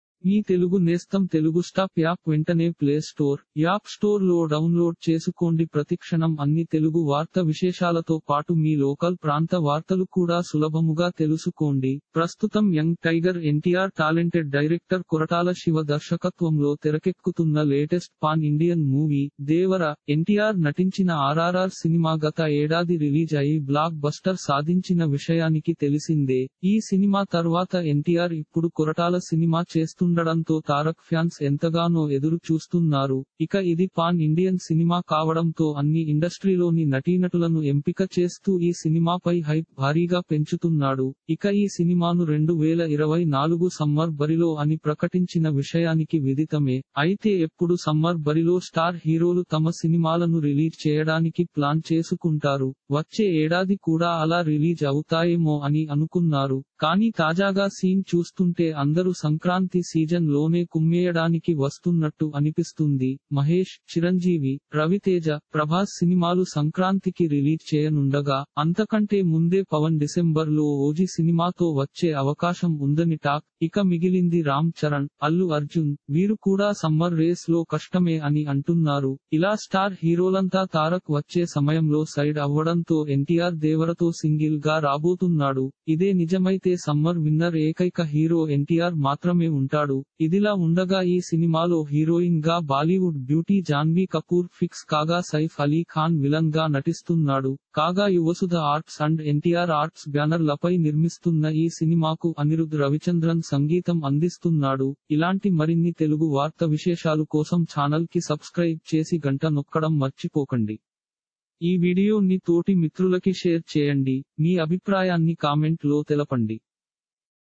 NTR Devara Movie Release In 2024 Summer - 'దేవర'తో ఎన్టీఆర్ సింగిల్ గా రాబోతున్నాడా.. సైడ్ ఇచ్చేసి
ప్రస్తుతం యంగ్ టైగర్ ఎన్టీఆర్ టాలెంట - NTR Devara Movie Release In 2024 Summer $telugu-title:'దేవర'తో ఎన్టీఆర్ సింగిల్ గా రాబోతున్నాడా.. సైడ్ ఇచ్చేసిన స్టార్ హీరోలు!$ *pid:2548398* https:/ - Telugu Movies TeluguMovie Tollywood TollywoodNews TeluguMovieNews Telugu TeluguStop | Movie